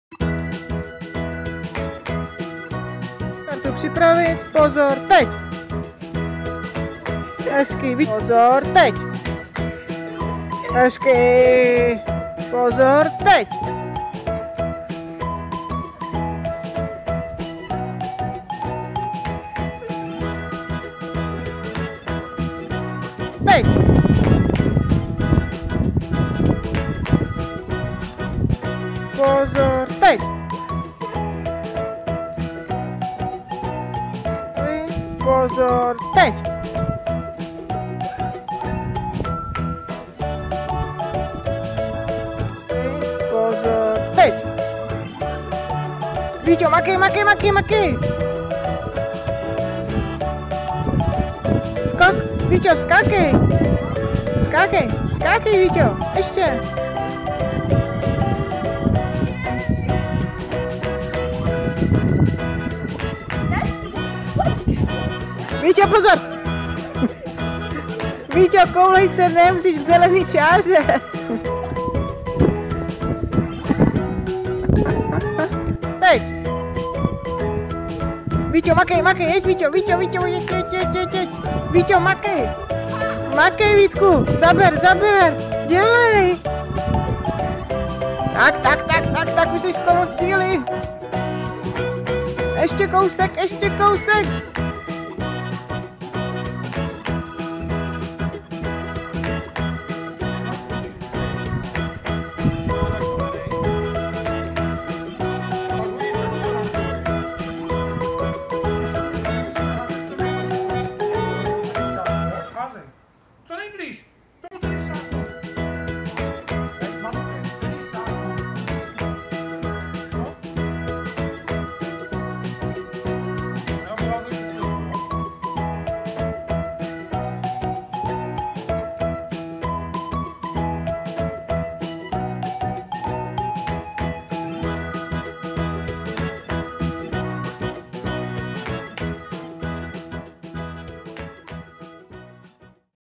Karanténa Bez Bariér - 6. Starty z poloh.
Vážení přátelé, dnes jen krátký text. Video mluví samo za sebe.

Děti si trošku zaběhaly, procvičovaly rychlost reakce, zrychlení a sprint na krátkou vzdálenost. Bavili se oba. Může se zdát, že toho malého ten větší "drtil", ale ten menší mu to o den dřív nandal 10 : 0. Fantazii se meze nekladou, berte video jen jako ilustrační, jistě se vám podaří vymyslet spoustu dalších startovních pozic..

Tak sportu zdar, atletice zvlášť. Vaši atleti Bez Bariér.